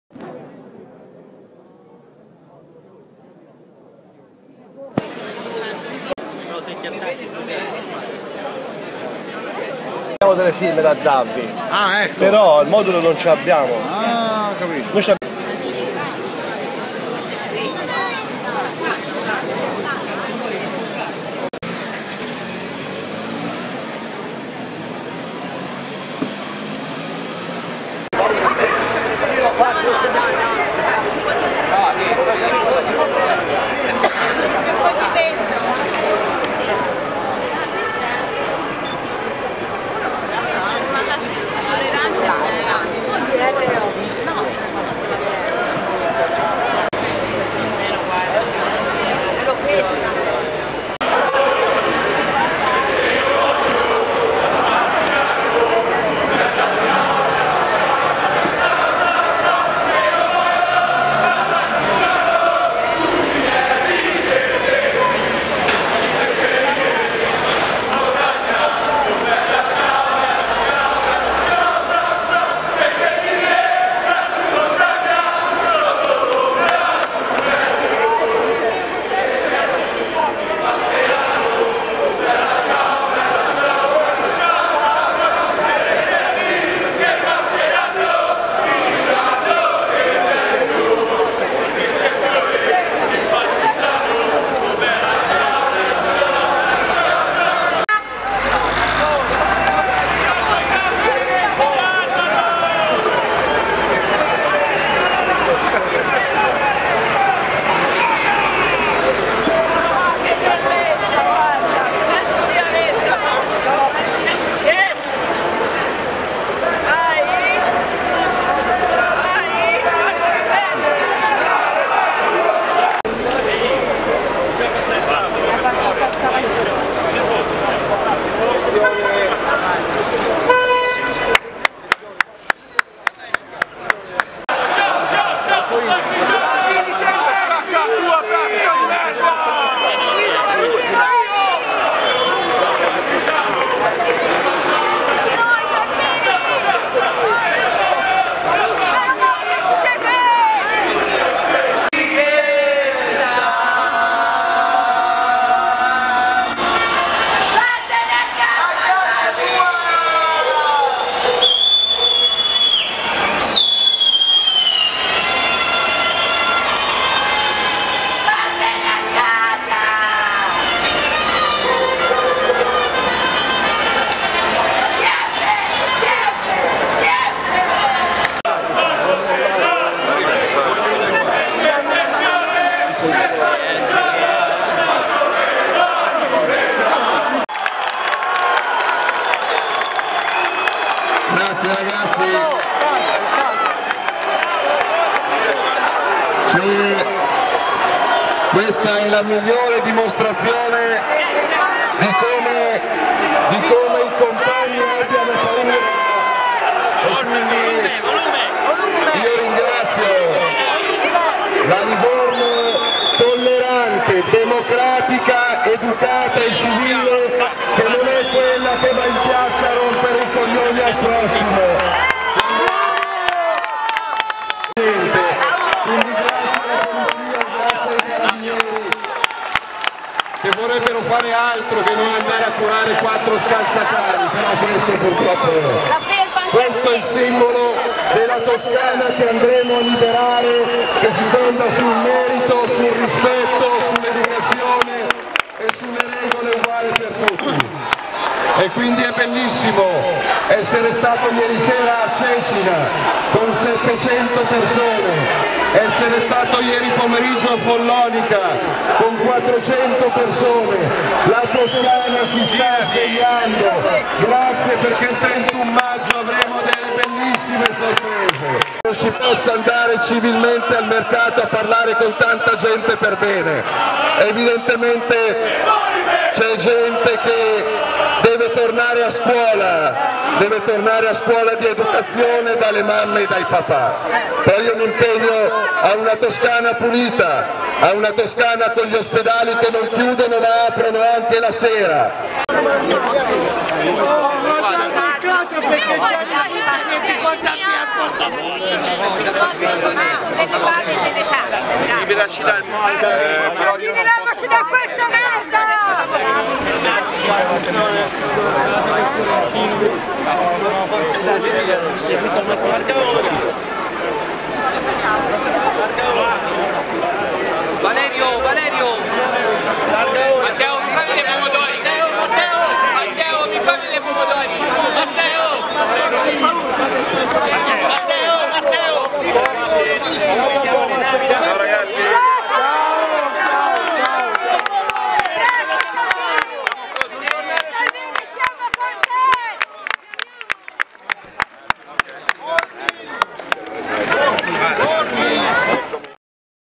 Matteo Salvini, comizio e contromanifestazione
Livorno, Matteo Salvini, comizio e contromanifestazione. Abbracci, fischi, uova e maglia del Livorno
Video di Alberto Vincenzoni / fotonewslivorno.blogspot.it